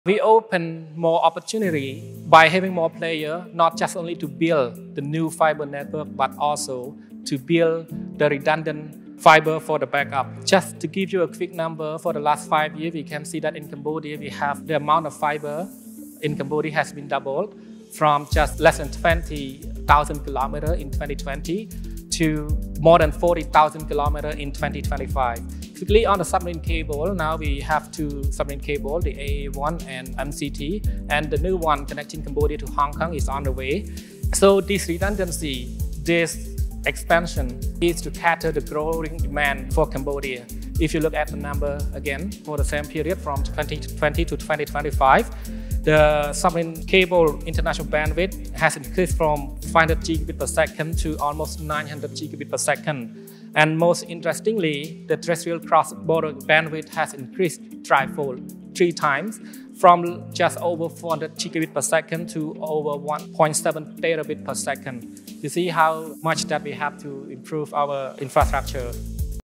Growing Cambodia's Fibre Network - Bunboren Tep, TRC
Bunboren Tep, Director of the Telecommunications Regulation Bureau of the Telecommunication Regulator of Cambodia (TRC), shared information on the expansion of Cambodia's fibre network over the last five years.

The fibre optic cable network in Cambodia has doubled from under 20,000 km in 2020, to more than 40,000 km in 2025. Two submarine cable systems are already established, while a new one connecting Cambodia to Hong Kong is on the way.

Bunboren delivered these remarks during a panel discussion on 'Infrastructure for Growth - Cloud, Connectivity, and Data Hubs' held on Day 3 of the Digital Government Forum 2025, which took place from December 5-7 at the Diamond Island Convention and Exhibition Centre.